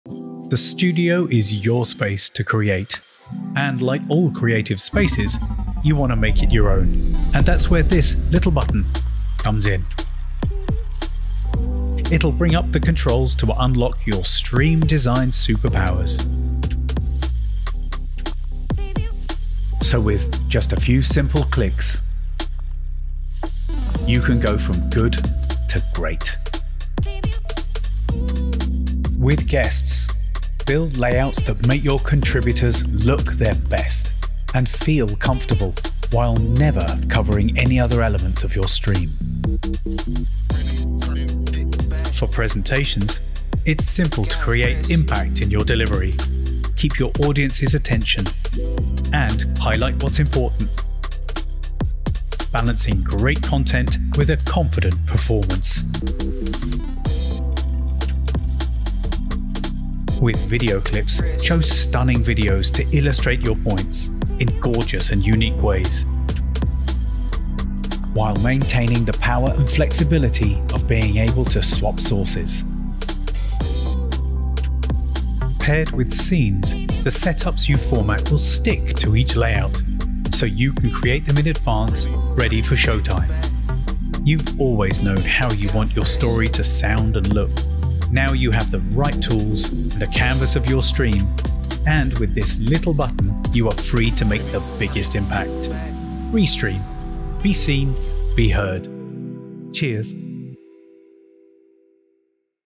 Custom Layouts in Restream Studio
Design your stream however you want with Custom Layouts in Restream Studio.

- Resize, reshape, and align your guests.
- Showcase media sources in a new way.
- Your adjustments "stick" to scenes for smooth, professional live videos.

Creating unique shows is now easy and free for all.